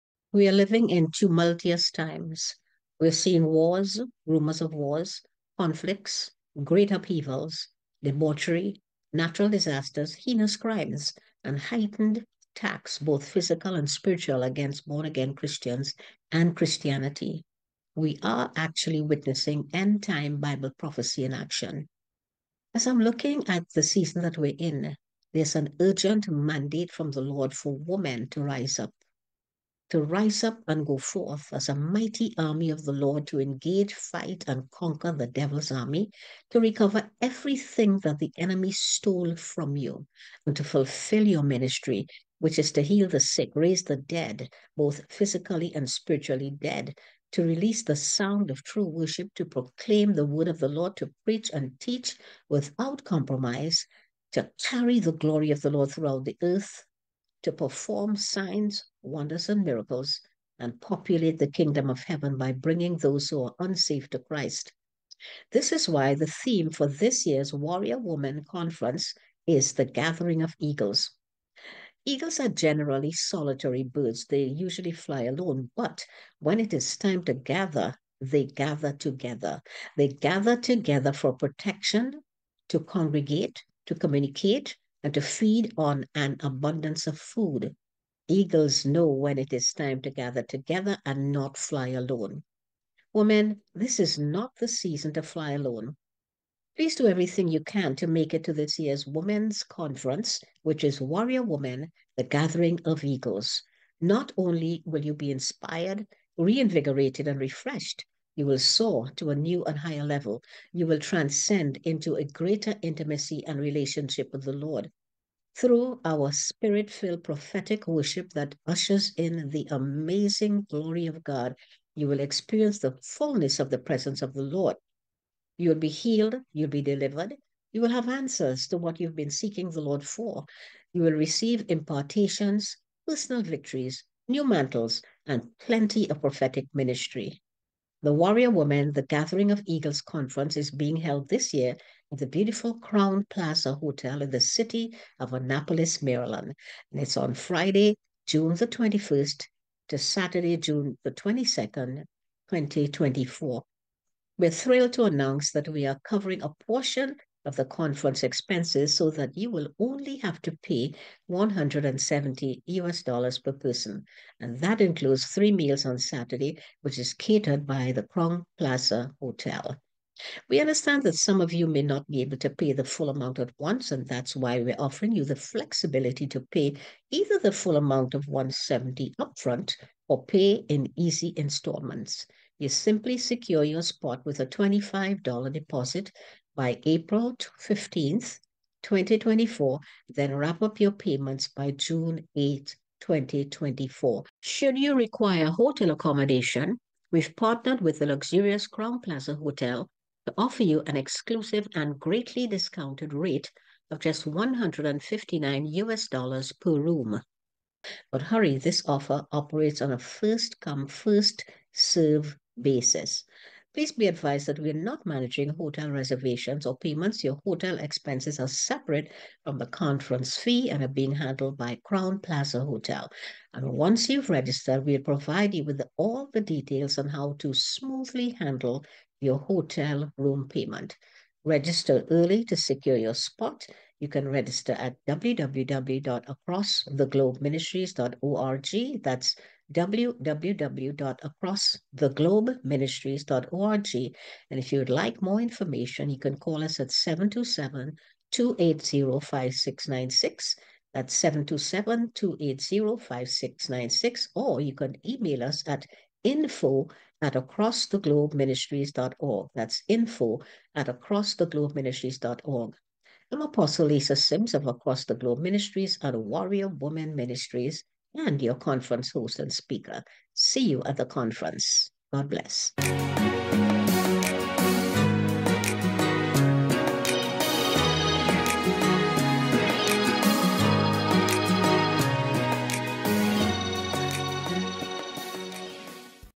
2024 Warrior Women : The Gathering of Eagles -June 21st-June 22nd, 2024
Please do everything you can to make it to this year’s Warrior Women : The Gathering of Eagles conference. Not only will you be inspired, reinvigorated and refreshed, you will soar to a new and higher level.  You will transcend into a greater intimacy and relationship with the Lord. Through our spirit- filled prophetic worship that ushers in the glory of God, you will experience the fullness of the presence of the Lord. You will be healed and delivered. You will have answers to what you have been seeking the Lord for. You will receive impartations, personal victories , new mantles and plenty of prophetic ministry. The Warrior Women The Gathering of Eagles conference is being held this year at the beautiful Crowne Plaza Hotel in the city of Annapolis Maryland on Friday June 21st - Saturday June 22, 2024.
Women only please!
Register to secure your spot at www,acrosstheglobeministries.org